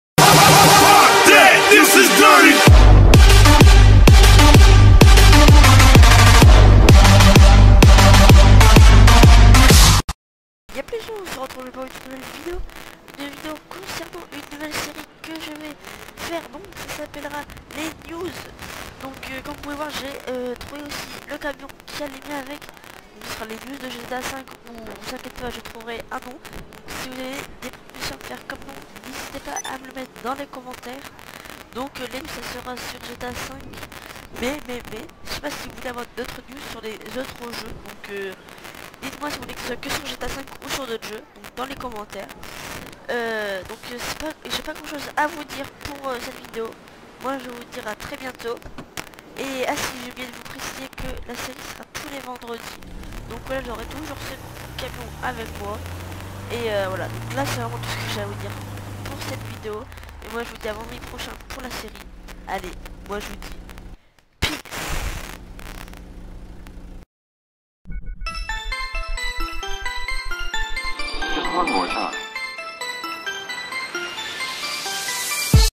new serie dite ce que vous en penser
DESCRIPTION IMPORTANTE
Yop les gens on se retrouve aujourd'hui pour une toute nouvelle vidéo ou aujourd'hui je vous propose une nouvelle serie les new de gta5 et la question si vous vouler que des infos gta5 ou autre jeu reponder dans lka barre des com si cette vidéo vous a plu n'hésiter pas a liker,commenter et partager pour plus de vidéo abonner vous bissssssssssssssssssssssssssssssssssssssssssssssssssssssss
XBOX one: play fayse PgM